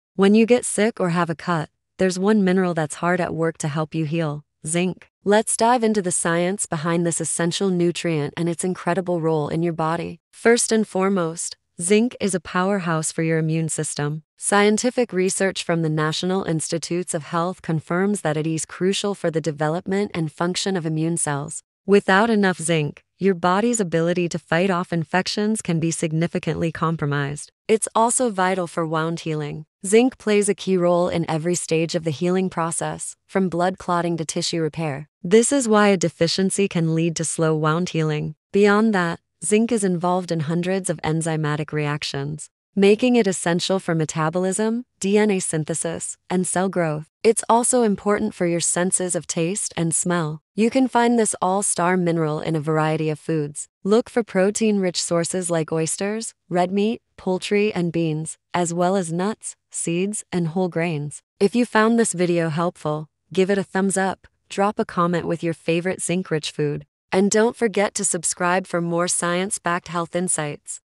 Zinc: The Immune-Boosting Mineral for Healing & Health
Feeling under the weather or just had a cut? You might need more zinc! In this video, we'll explore the incredible science behind this essential mineral.
You’ll discover its crucial role in immune function and wound healing, why it's vital for metabolism, and how it supports your senses. We’ll also give you a list of the top zinc-rich foods to add to your diet.